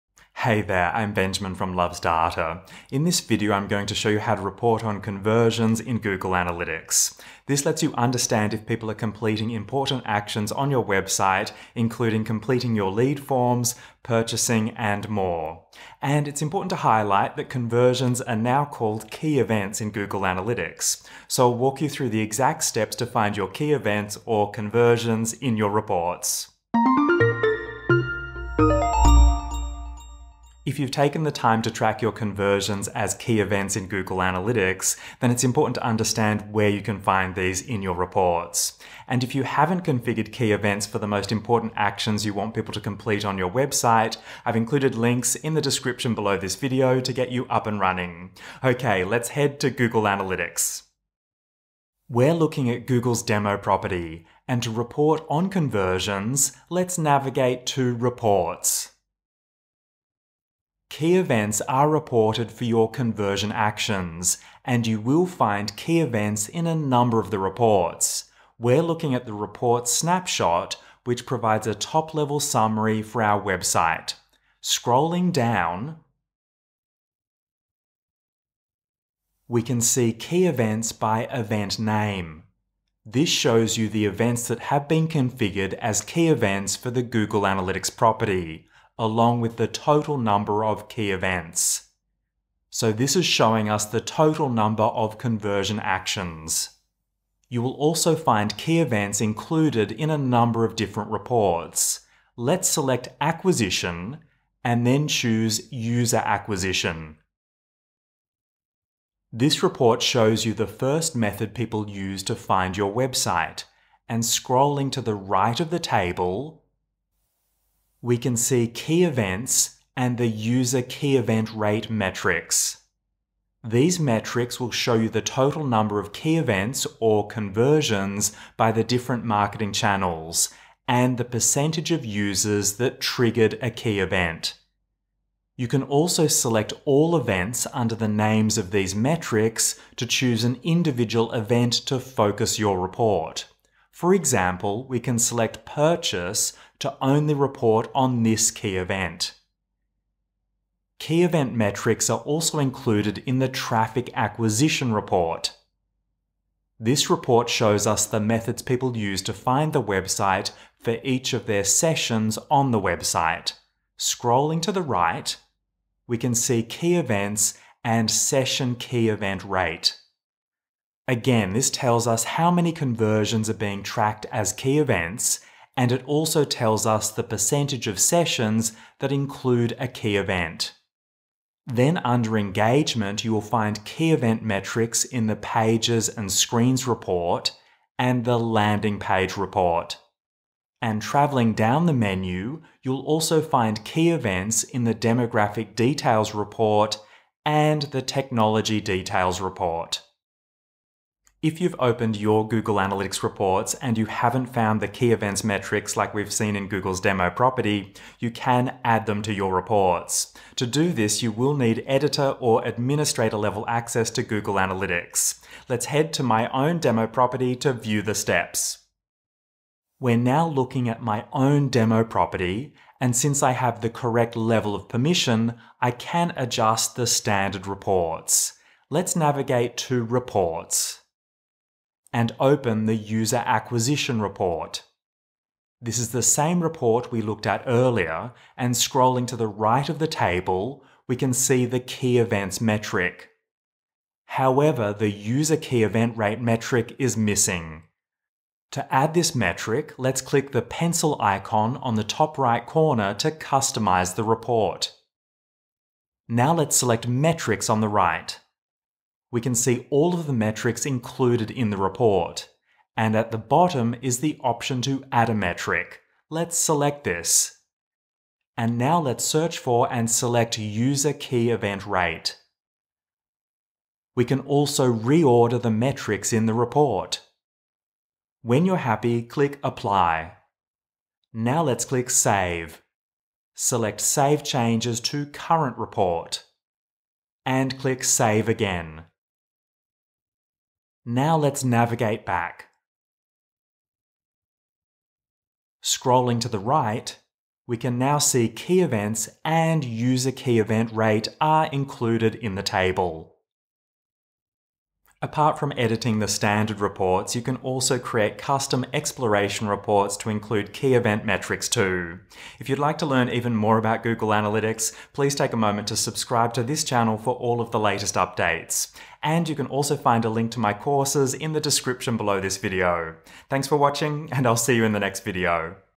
Report Conversions Using Key Events in Google Analytics 4
Learn how to report on conversions using Key Events in Google Analytics 4 (GA4). Walk-through the steps to find the default conversion metrics, including 'Key Events', 'User Key Event Rate', and 'Session Key Event Rate' in the standard reports. Plus, how to edit the standard reports to add key event metrics if they're missing from your reports. Let's jump in and cover how to report on conversions using Key Events in Google Analytics!

JUMP TO A TOPIC:
00:00 Introduction to Key Events
00:51 Viewing Key Event metrics
03:39 Edit reports to include Key Event metrics
05:04 Next steps